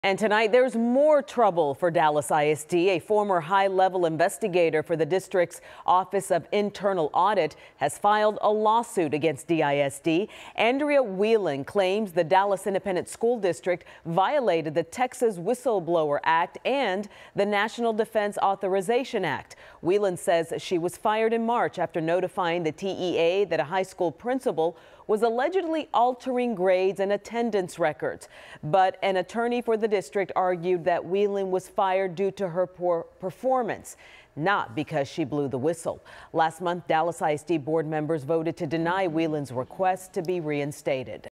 Former Dallas ISD auditor suing district
Andrea Whelan claims Dallas ISD violated the Texas Whistleblower Act and the National Defense Authorization Act.